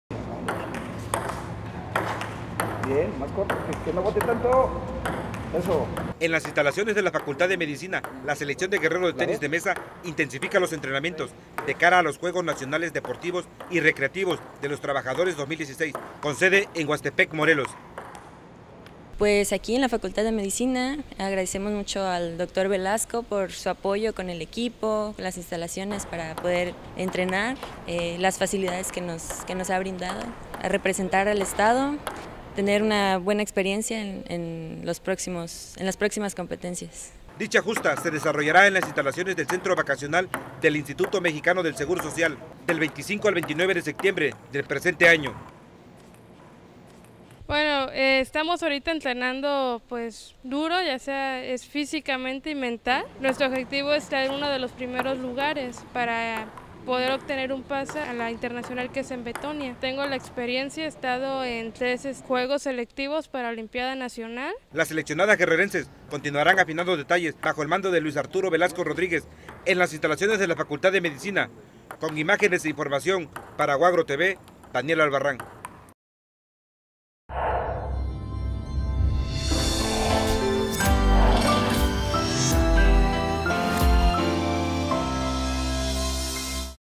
Tenis de Mesa Selección Guerrero
Alumnas de la UAGro representaran a Guerrero en los 44 Juegos Nacionales de los trabajadores.